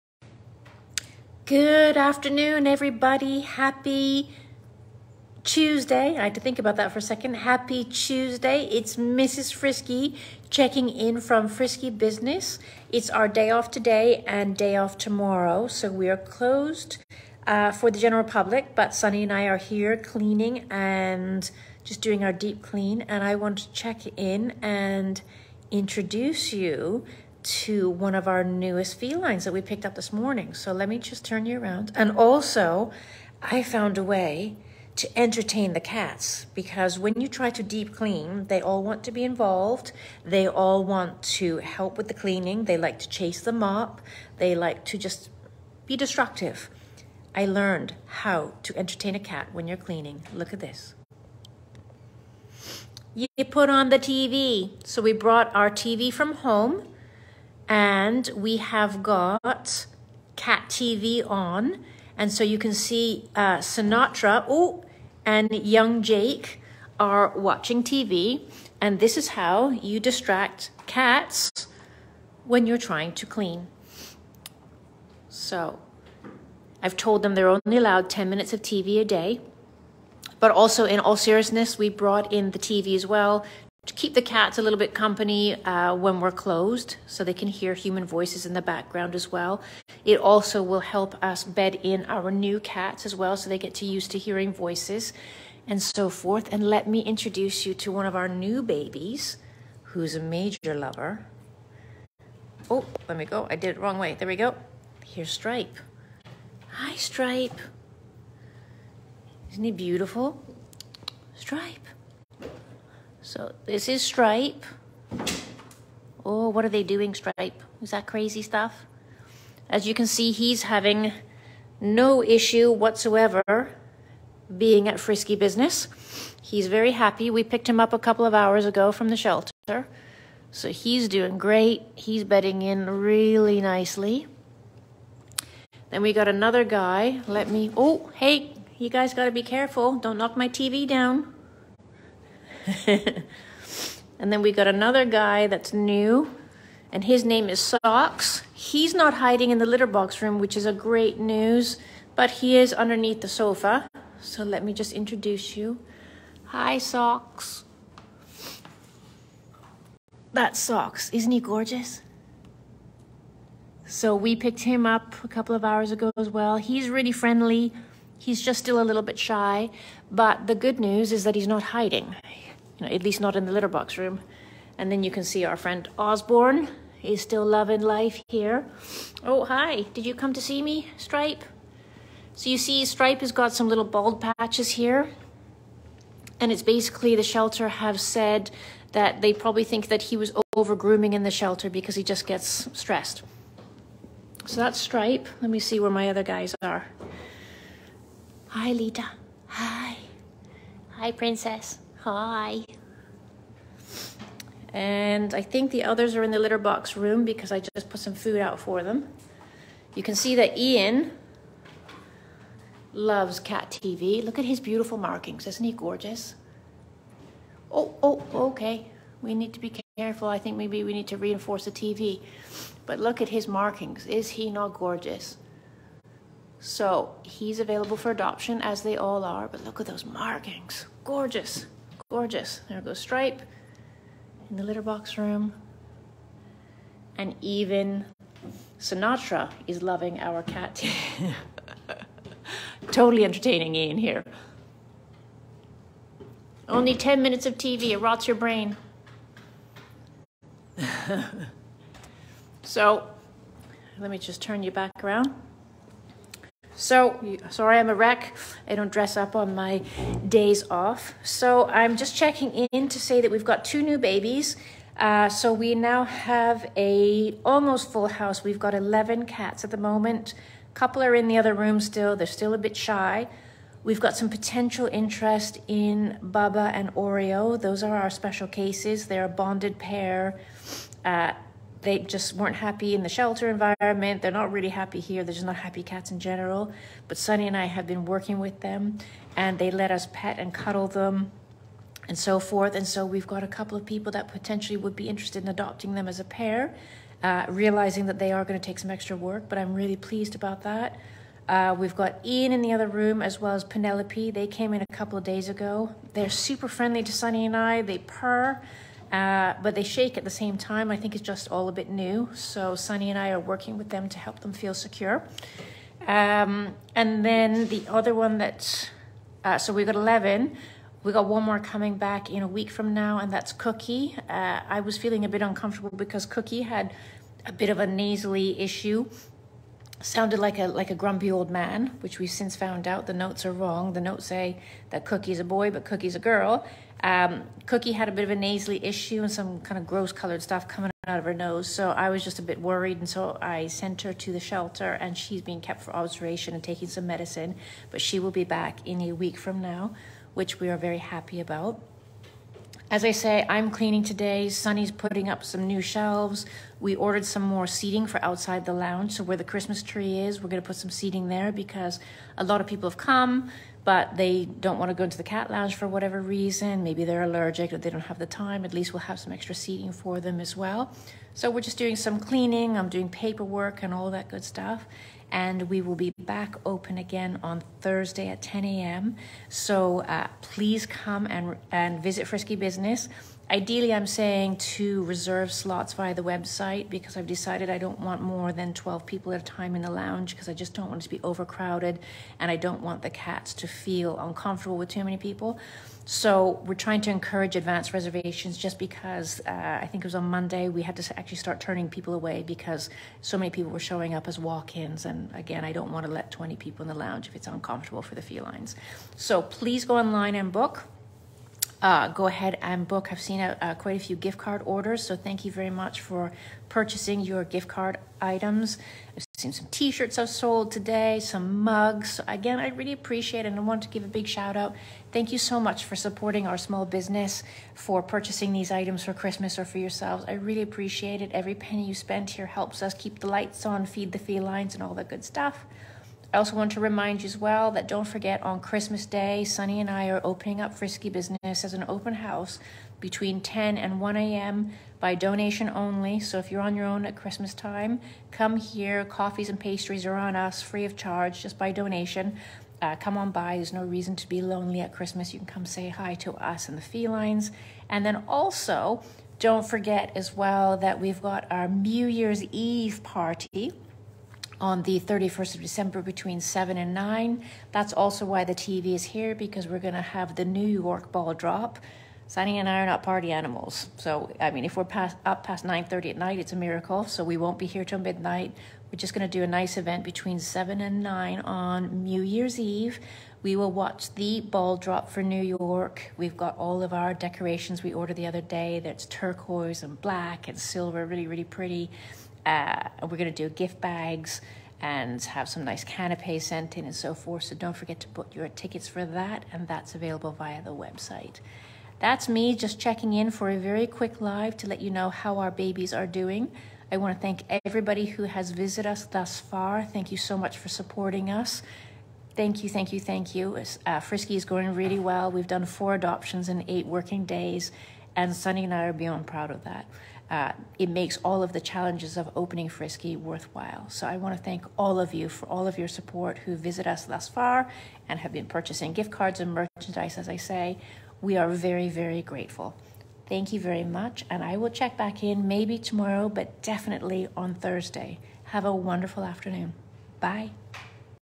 Frisky Update
Claire gives another update from Frisky Business Cat Cafe

Frisky Business Links